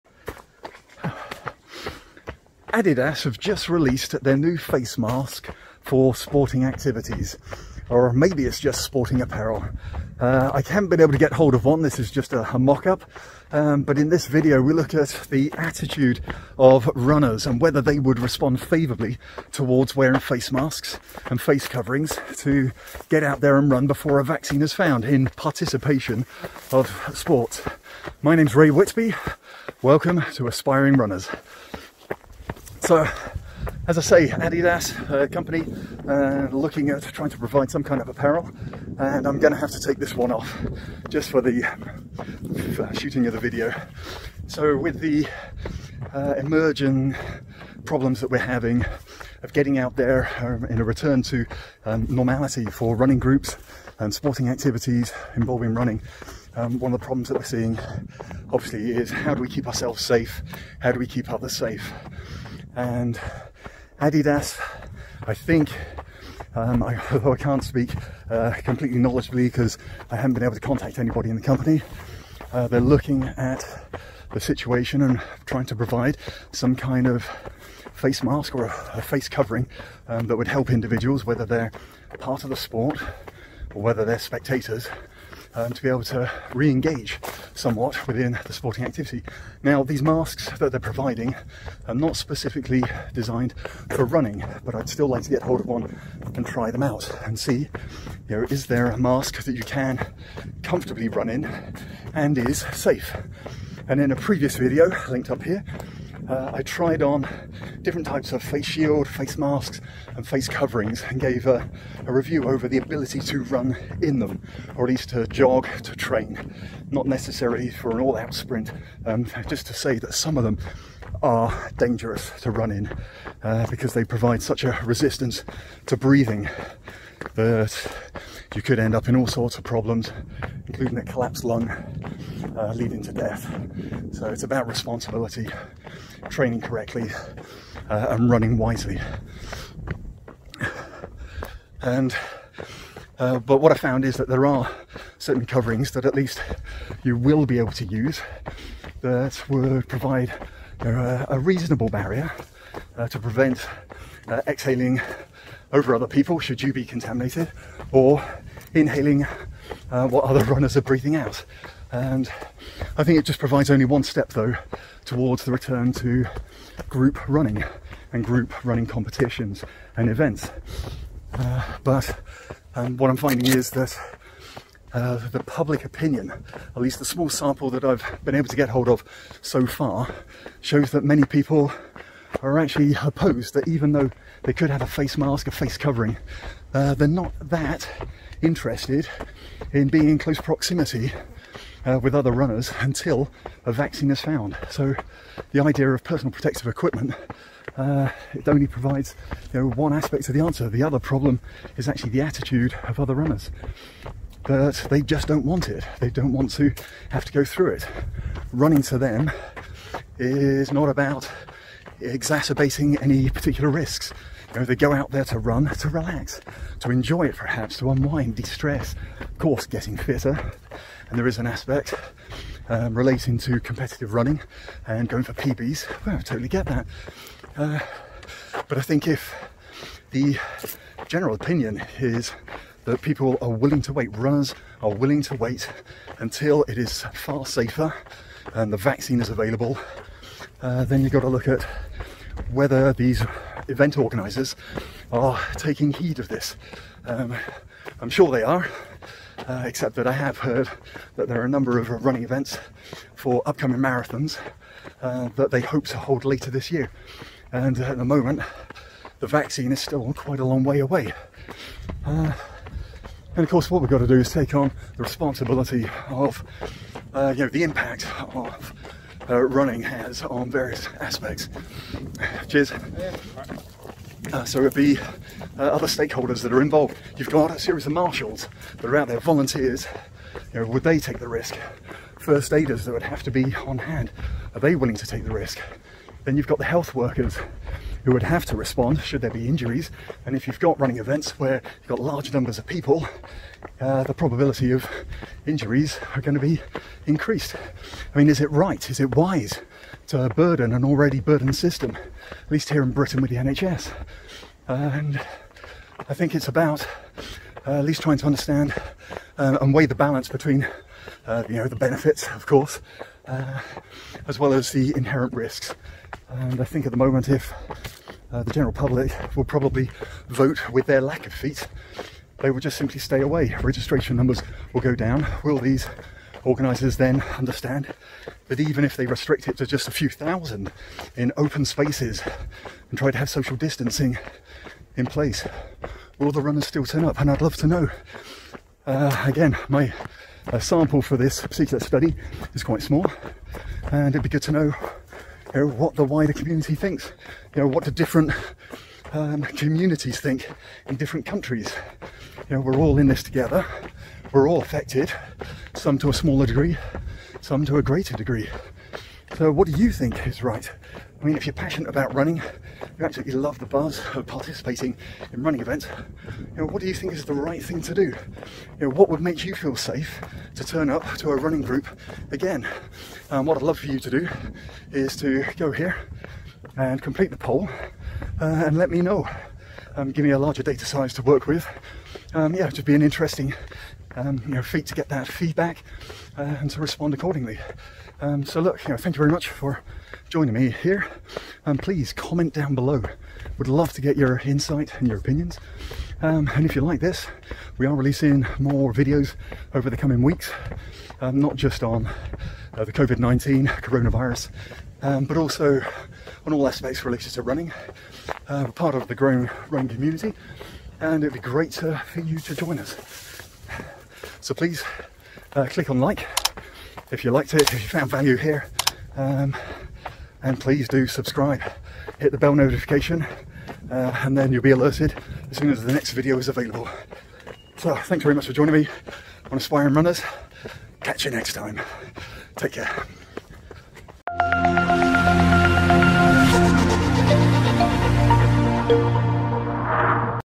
Is it safe to race in 2020? (Feedback from runners on coronavirus and face masks)
A number of running event organisers are moving ahead with registration for upcoming road and trail marathons before a coronavirus vaccine is available. The question is do you feel a running event could be made safe enough for you to run, whether using face masks or face coverings and safe operating procedures?

When looking at the updates from organisers it is clear that safety is their number one priority and they are monitoring the situation closely. And they feel with restricted numbers, wide open spaces, perhaps the event could go ahead.

I have asked a variety of runners what would it take to make them feel safe enough to participate in a running event where gatherings and groups of runners are likely to form. But, this is a small sample and I would love to have your input. Please watch through to the end and complete the poll.

Having tested a few face masks and face coverings for running, they may provide a reasonably safe protective barrier (if used correctly), but I think safety equipment does not go far enough to allay the risks. So even if manufacturers (see Adidas face mask) can provide a safe to run in face mask, it may be that running events will have limited participants.

Many people love running, especially trail running, and hoping they can participate in a marathon later in 2020, but are still hesitant to take on risks that coronavirus, covid 19, present. What do you think?

⏱ Timestamps ⏱
0:00 Is it safe to race in 2020? (Feedback from runners on coronavirus and face masks)
0:30 Introduction
6:15 The Poll